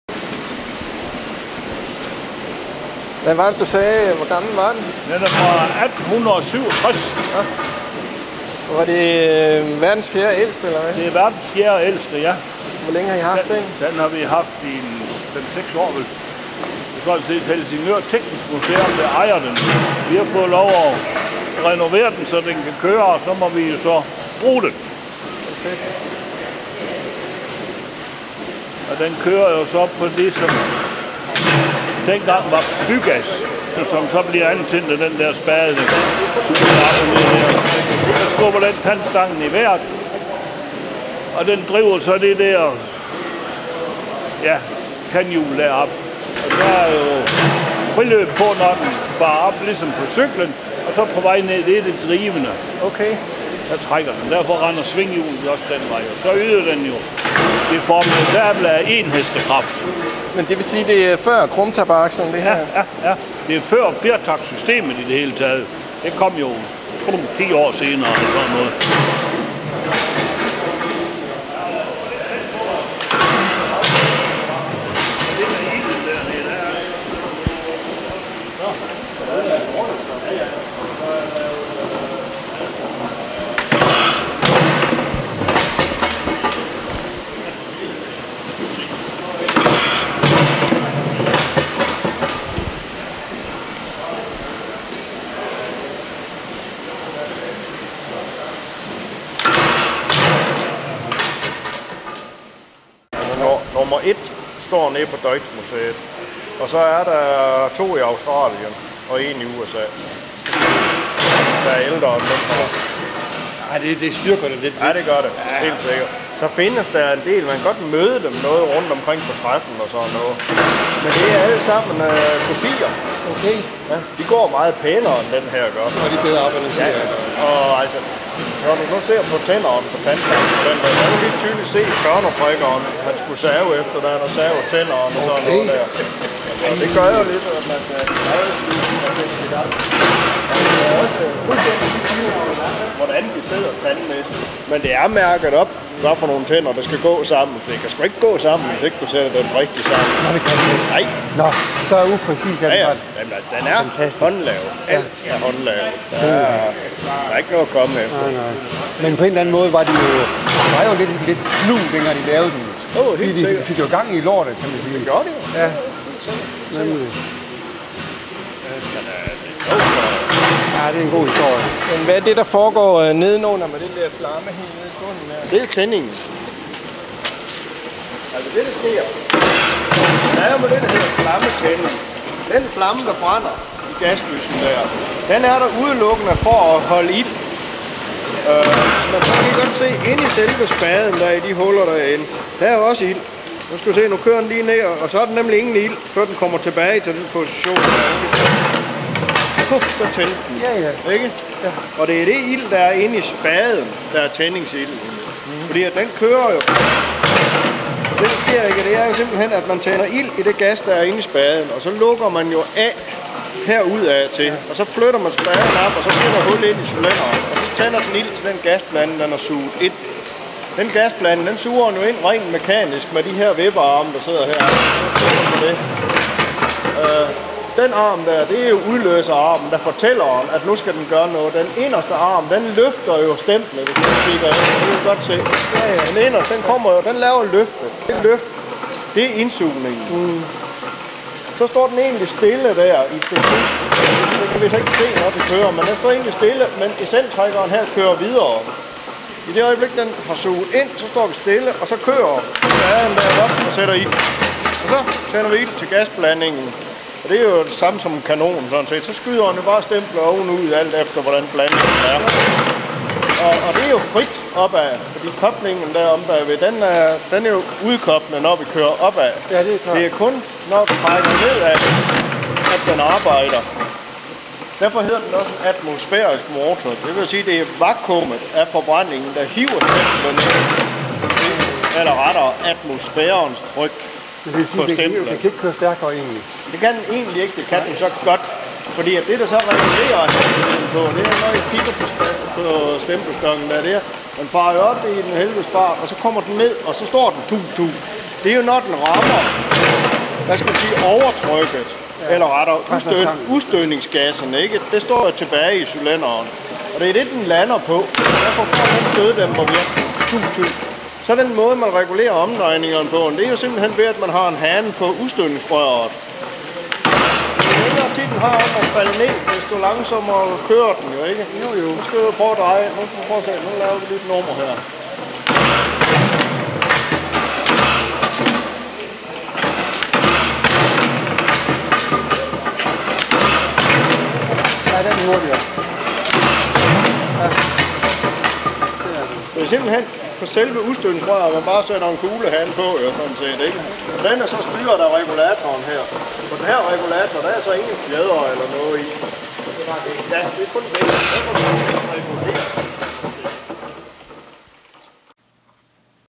Worlds 5. Oldest Combustion Engine - 1867(!) Langen-Otto/ The Engine Collection
A non-crankshaft precursor to the 4-stroke engine. A cogged rod is shot,pulling on the way down. 1 HP, town gas.